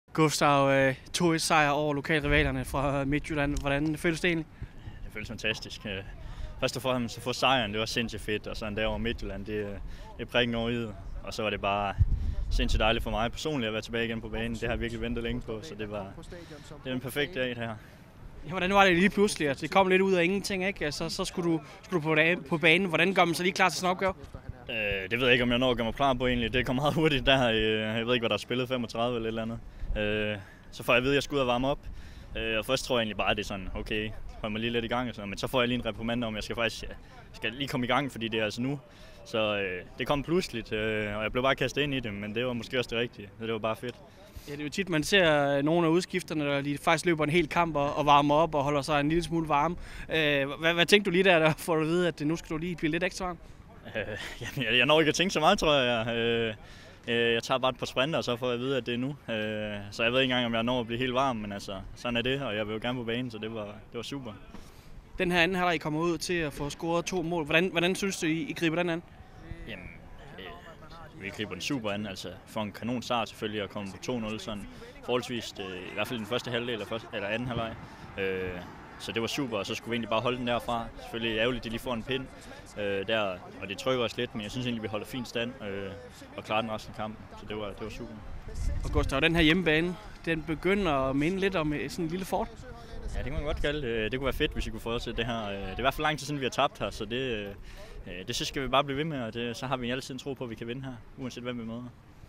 Overrasket Dahl: Var ikke helt varm
Gustav Dahl kom på banen efter 35 minutter i en taktisk udskiftning af Mikkel Cramer. Det kom dog lidt som en overraskelse for venstrebacken, som ikke var helt varm.